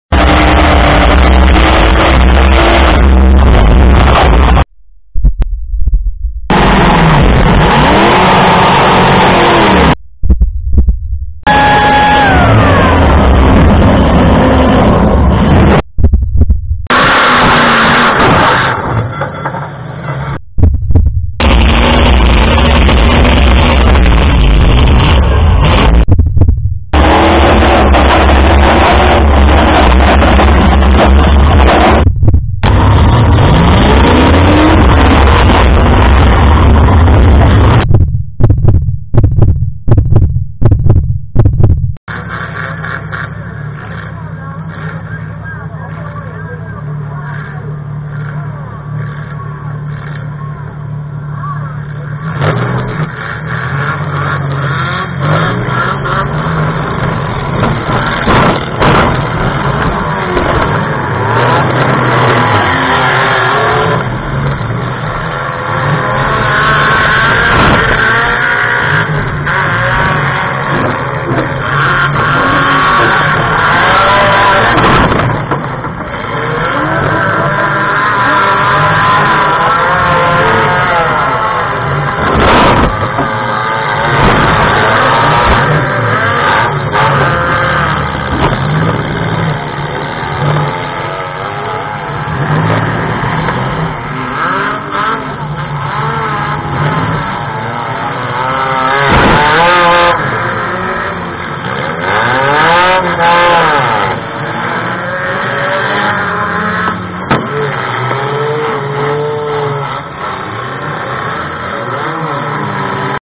P2 Compact Danville Fair 2022
Justin Jones P2 Compacts Vermilion County Fair Danville, IL 6/27/22